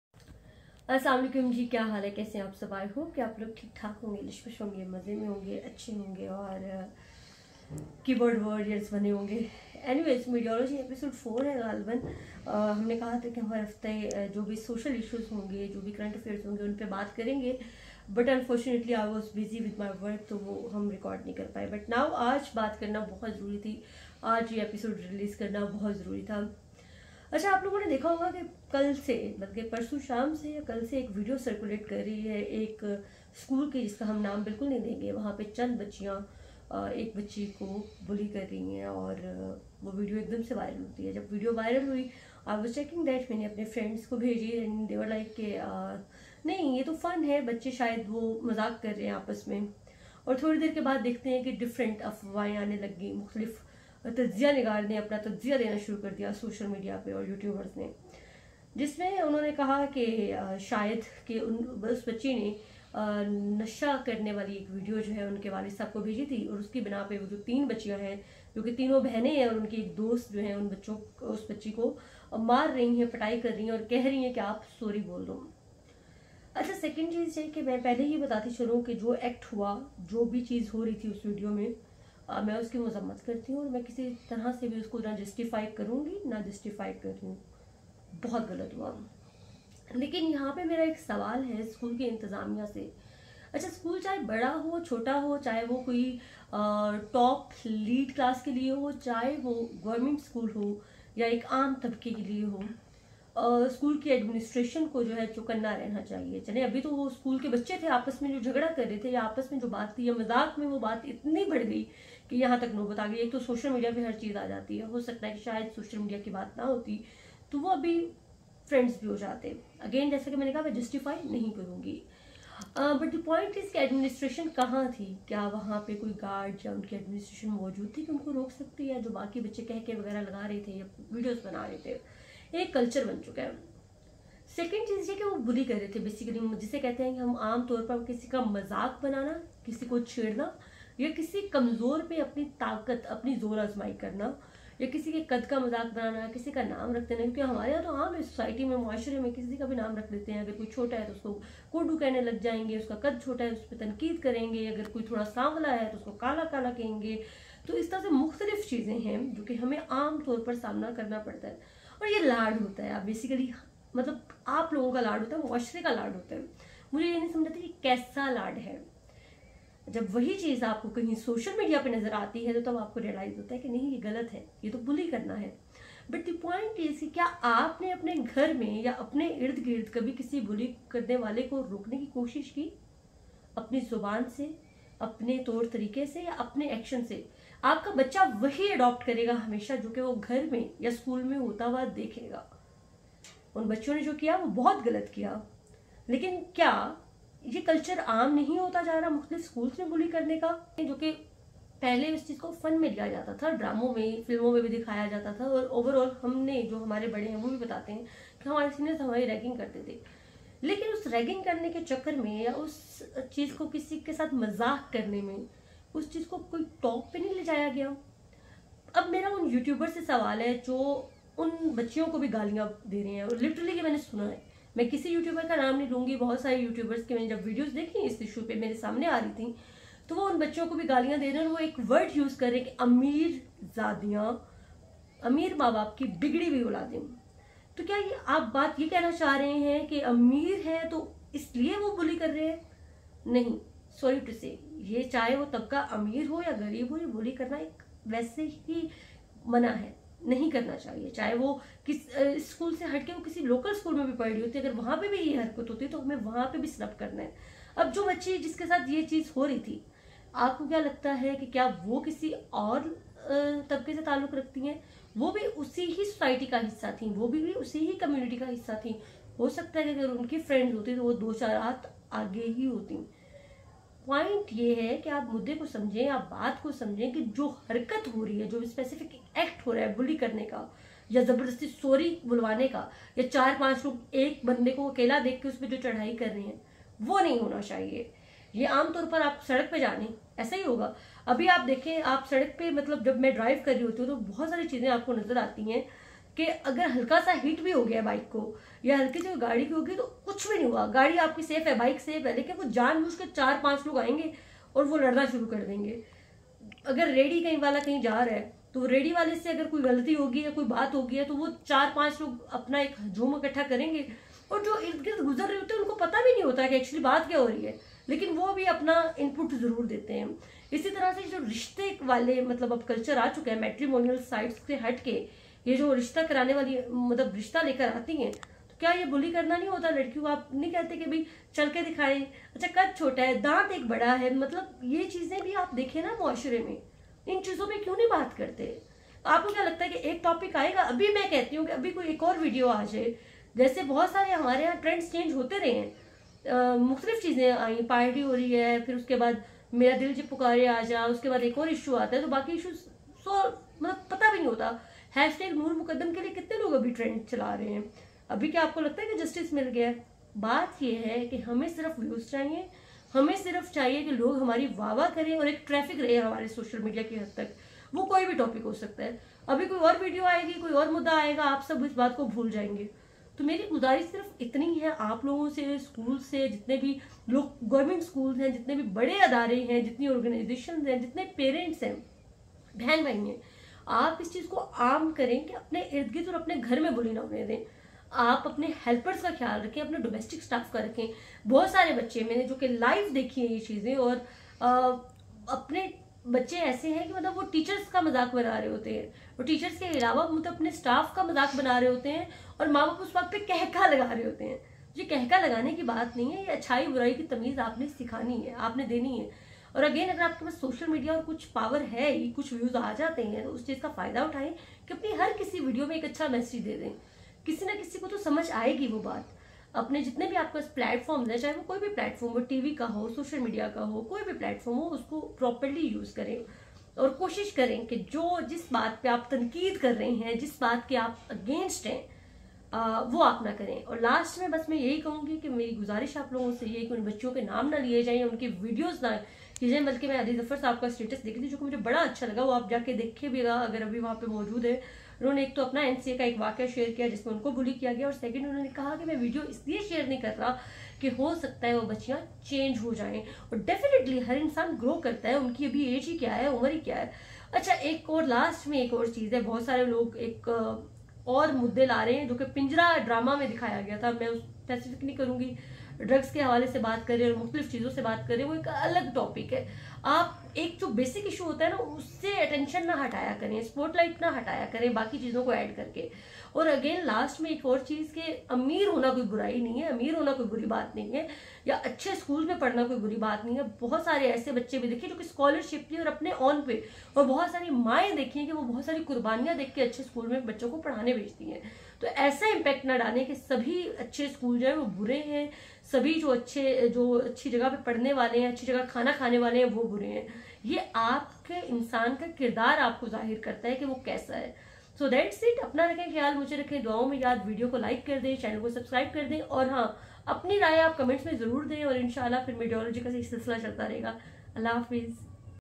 School Girl's Incident | Reality | Bullying
In this Episode I talked about incident of Girl's Fight in school.
Talked about Reality of that video.